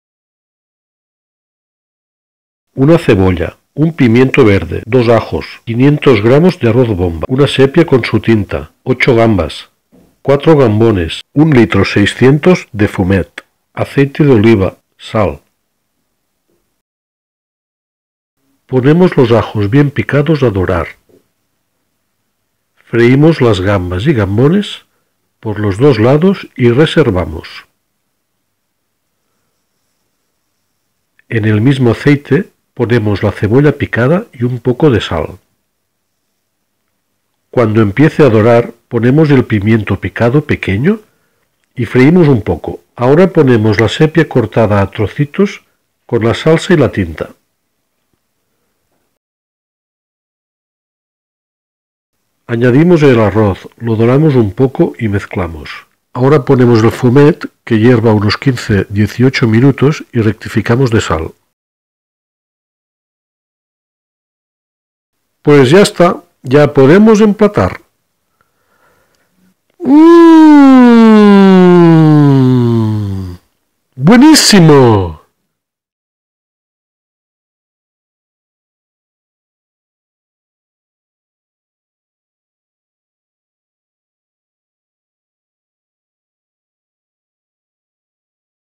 Arroz negro
Cómo hacer un arroz negro sin gluten bueniiiiiiiiissimmmo, próximamente os pondré la receta de cómo hacer un buen fumet de pescado (que hace falta para hacer este arroz negro)